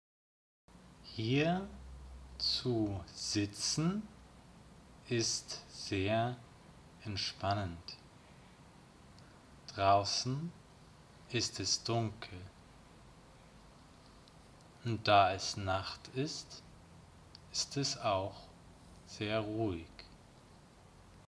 Nacht GermanForBeginners German Deutschlernen lerneDeutsch
▼ READ ME ▼

Ein ruhiges Video in der Nacht für Deutsch-Anfänger.

►► Transcript:

Hier zu sitzen ist sehr entspannend. Draußen ist es dunkel.
Und da es Nacht ist, ist es auch sehr ruhig.